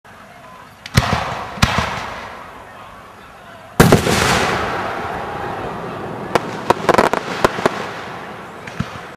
Pivoine a changements de couleur et crossettes vertes
Bombes aériennes de 102mm (4 pouces) de diamètre

Conception et réalisation par Artificier Frédérick Duguay